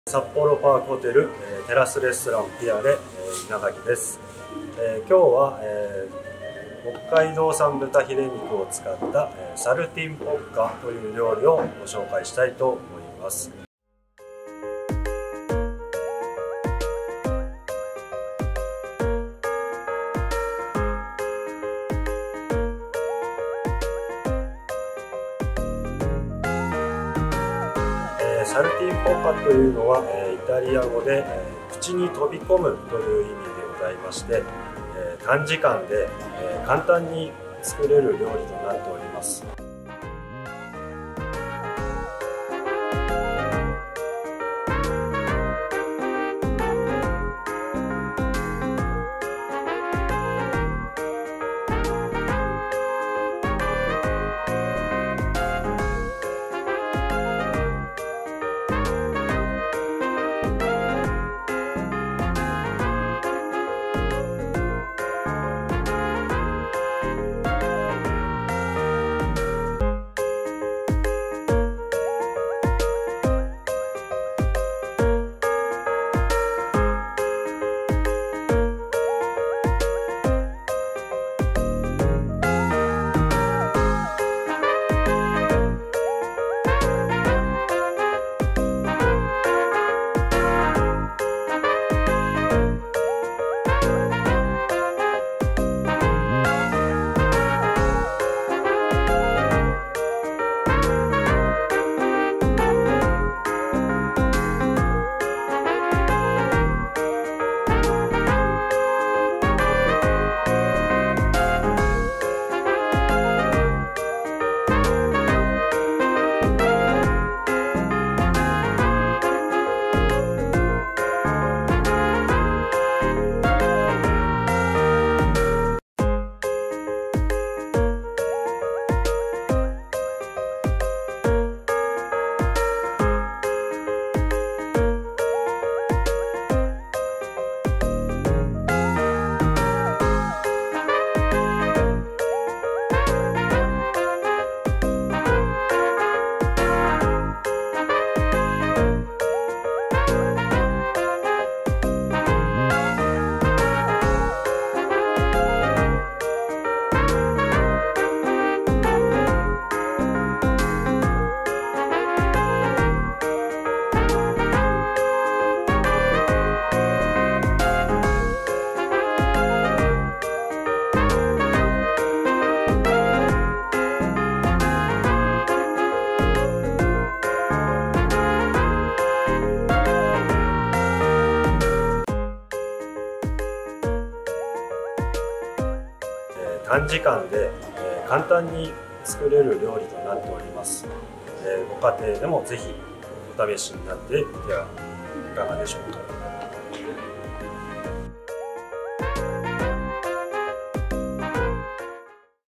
【公式】札幌パークホテル クッキングチャンネル#04「北海道産豚フィレ肉のサルティンボッカ」
札幌パークホテル　ピアレ調理長　稲垣幸治がおうちでできるホテルの味、第四弾「北海道産豚フィレ肉のサルティンボッカ」をお届けします。

■ レシピ（1人前）
北海道産豚フィレ肉　　　　　　　100ｇ（50ｇ×2）
生ハム　　　　　　　　　　　　　2枚
セージ　　　　　　　　　　　　　2枚
バター　　　　　　　　　　　　　30ｇ
白ワイン　　　　　　　　　　　　40ｇ
オリーブオイル　　　　　　　　　10ｇ
小麦粉　　　　　　　　　　　　　適量
塩　　　　　　　　　　　　　　　適量
コショウ　　　　　　　　　　　　適量
付け合せ（ズッキーニ、ジャガイモ、パプリカ、インゲンなど）

■ 作り方
1. 付け合せのジャガイモは茹でておく。その他の野菜をオリーブオイルで焼き、塩コショウをする。
2. 豚フィレ肉は50ｇにカットし、ラップではさみ肉タタキで薄く延ばす。
3. 豚フィレ肉の片面に塩コショウをして、もう片面にセージと生ハムを貼り付ける。
4. 豚フィレ肉に小麦粉をつける。フライパンにオリーブオイルとバターを熱し、セージと生ハムをつけた面から焼く。
5. 生ハムが色づいたら、ひっくり返し白ワインを注ぐ。
6. 豚フィレ肉に火が通ったら、フライパンから取り出し付け合せと共にお皿に盛る。
7. フライパンに残ったソースをかるく煮詰め、バターと塩コショウで味を整えソースを作る。
8. 豚フィレ肉にソースを流して完成。